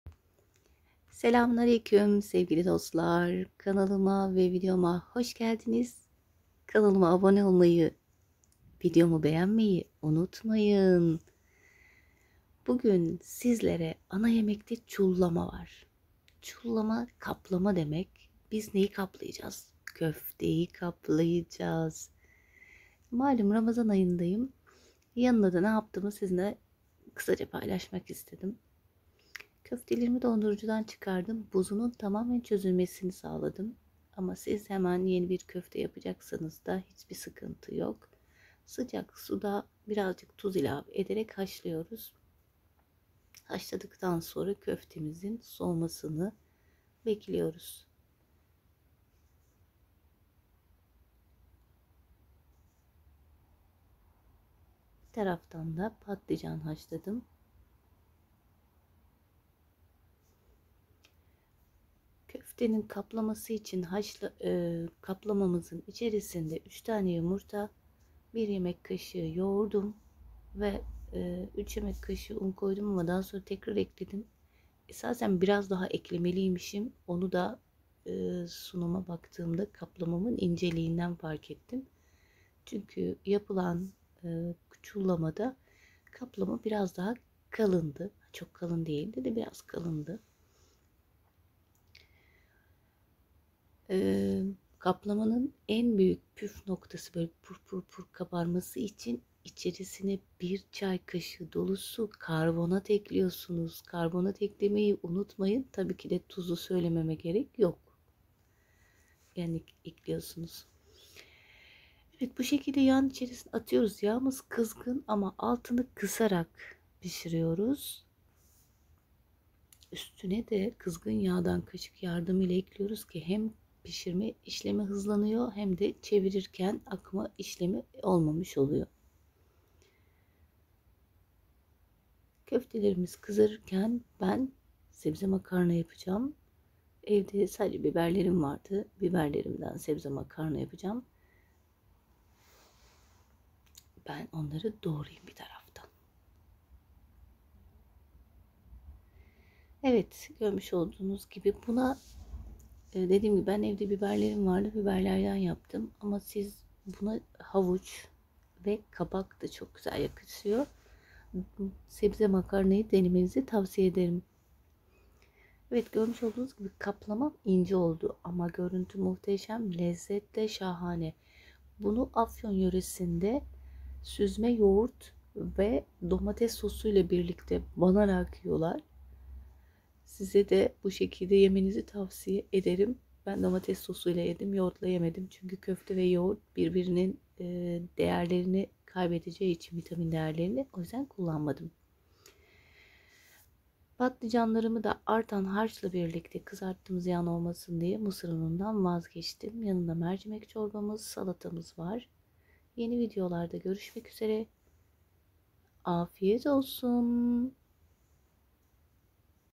ÇULLAMA Nasıl Yapılır? Yanına neler yaptım🥰
Afyon yönesine ait olan çullama, yoğurt veya domates sosu eşliğinde yenilir.